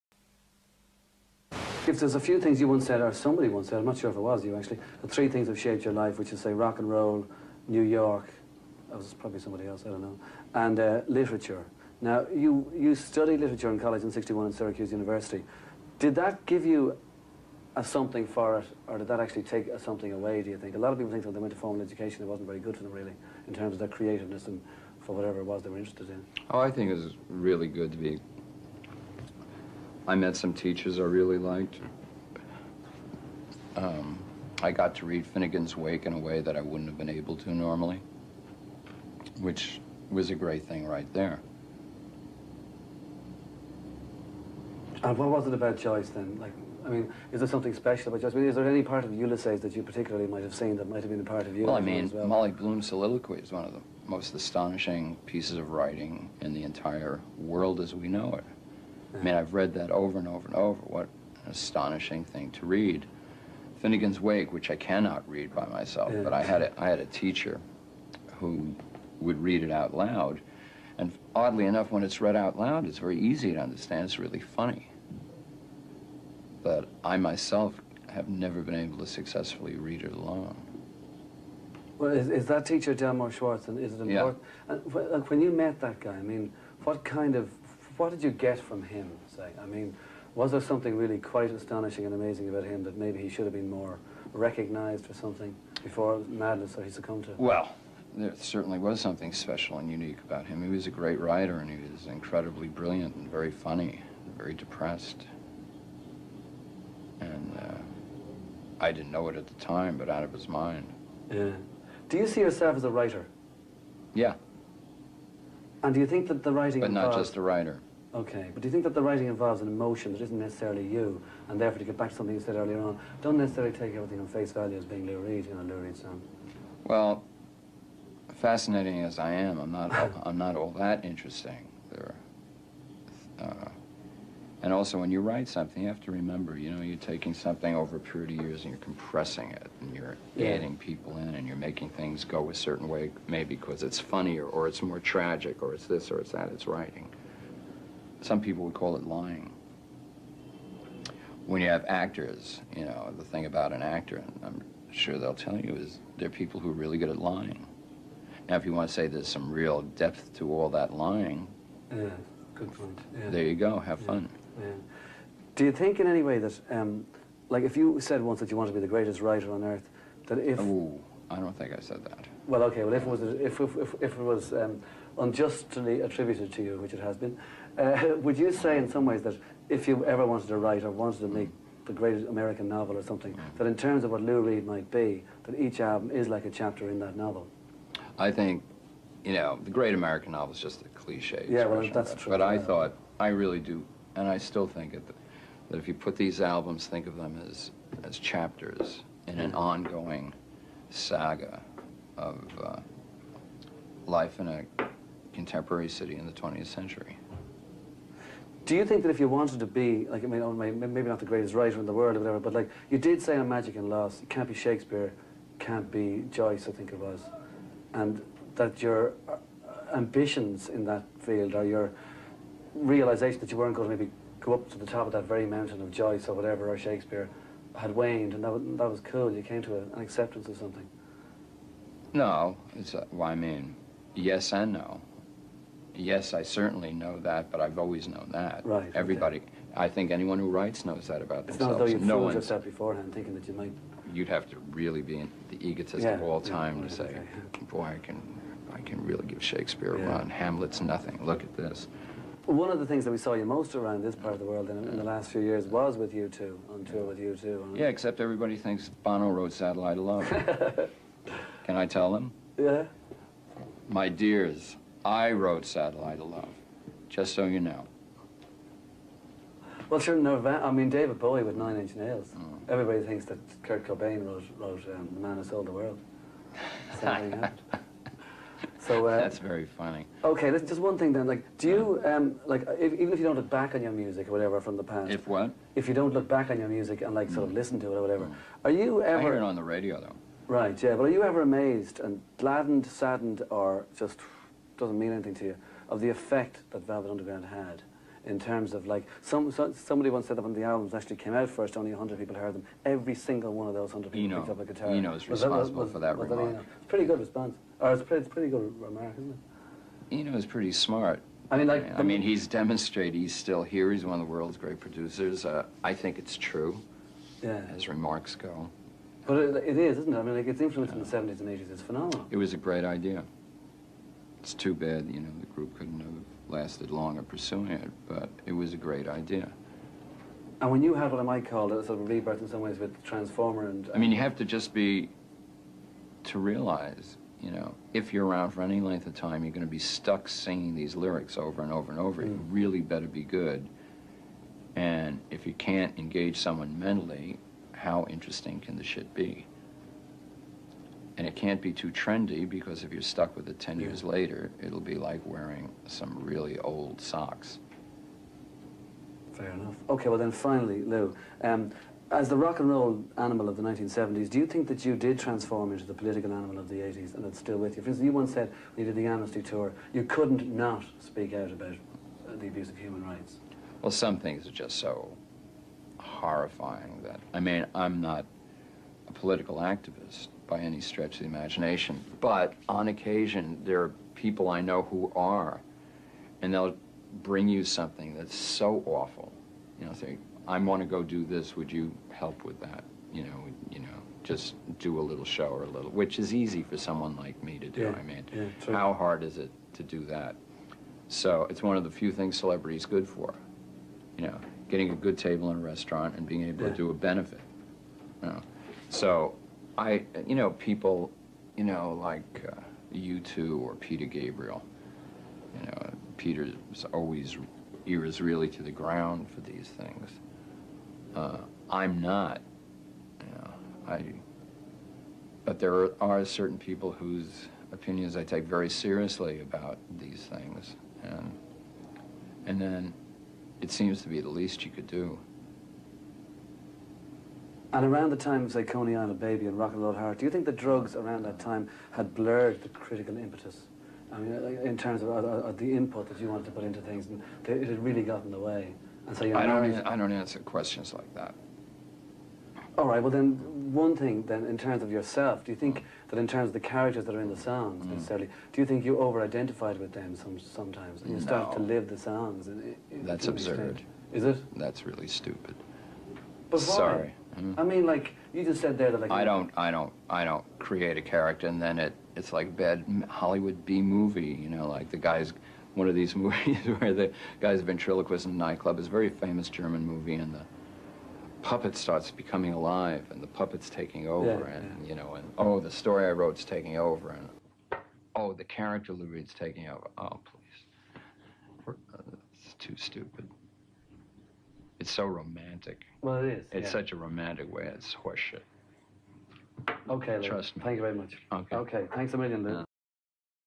Lou Reed gives a terse interview for Planet Rock
Lou Reed shows an intelligence matched by his ability to access his anger and frustration in this interview.